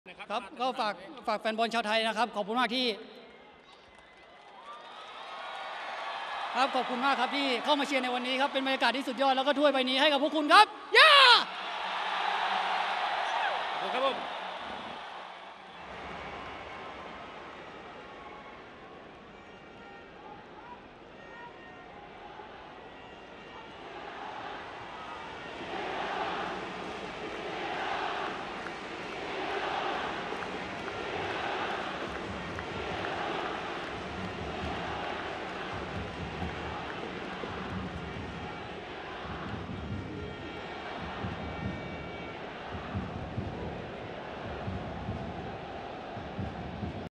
Theerathon phấn khích đáp lại sự hò hét của người hâm mộ
Startalk - Trang Thông tin video trực tuyến đầu tiên, cập nhật về Sao, về thể thao